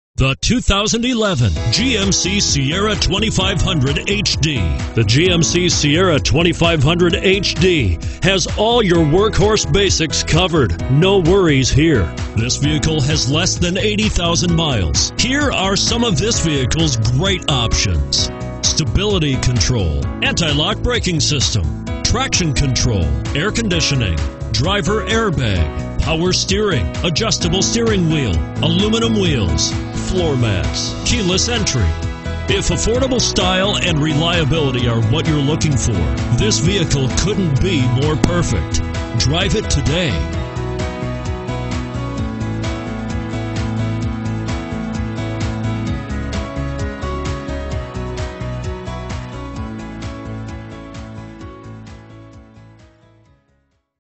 2011 GMC Sierra 2500HD Edmonton AB PT12914
2011 GMC Sierra 2500HD SLE

Southgate Buick GMC
9751 34 Avenue

Looking for the right car? Today could be your lucky day. With 75718 miles, this Black, 2011 GMC Sierra 2500HD equipped with Automatic transmission could be yours. Request more information and set up a test drive right away.